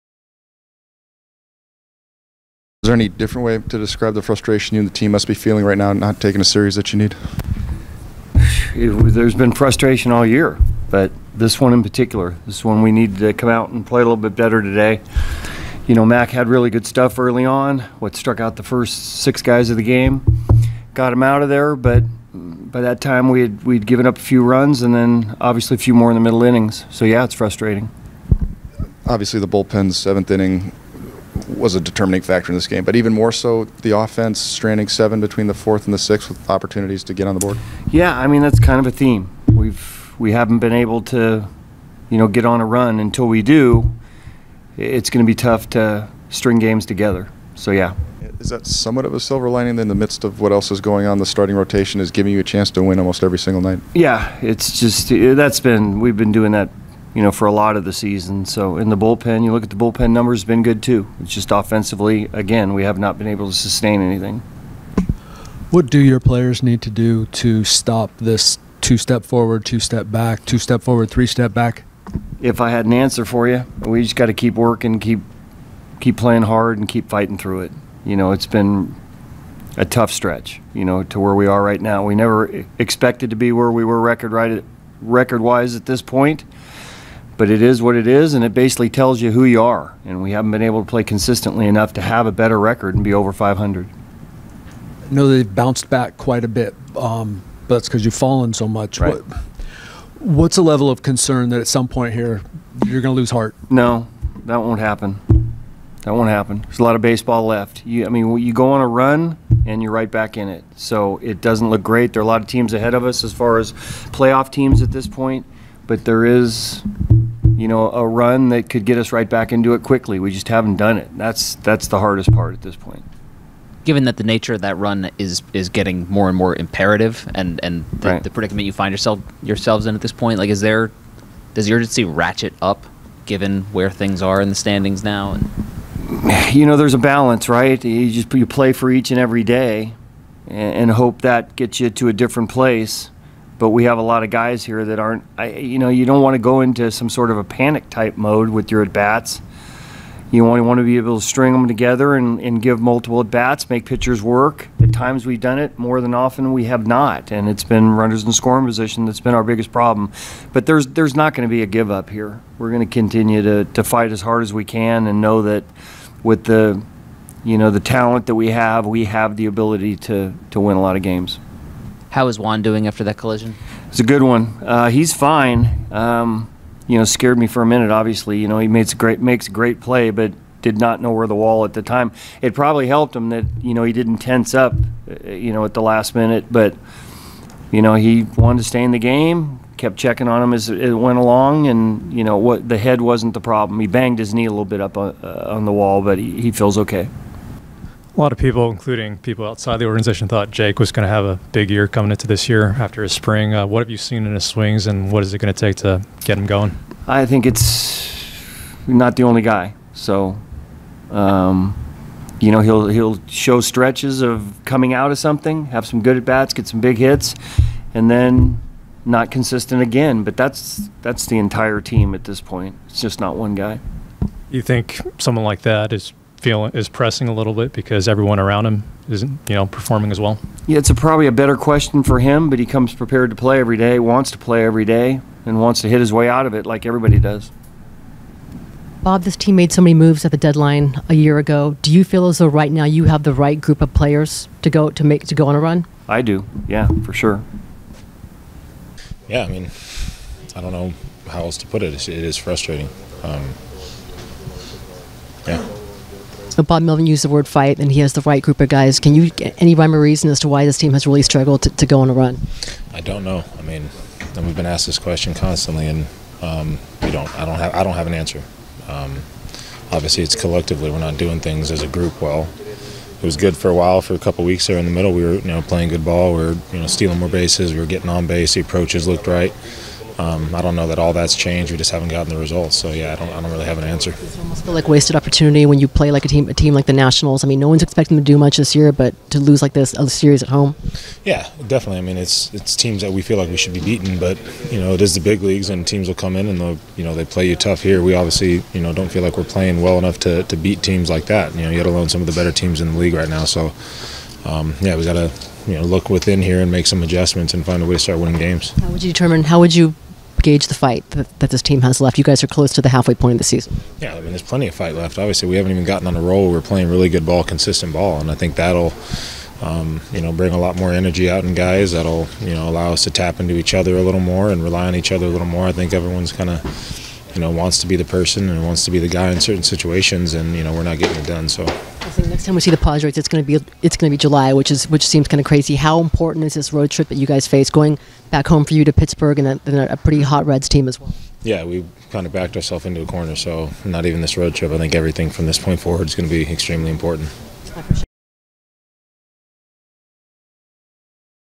Bob Melvin on Padres frustrating loss to Nats and why he says the Padres won't lose heart
Padres manager Bob Melvin discussed the high level of frustration after a series loss to the Nationals, why he says the team isn't going to lose heart and why he has belief the team can turn it around.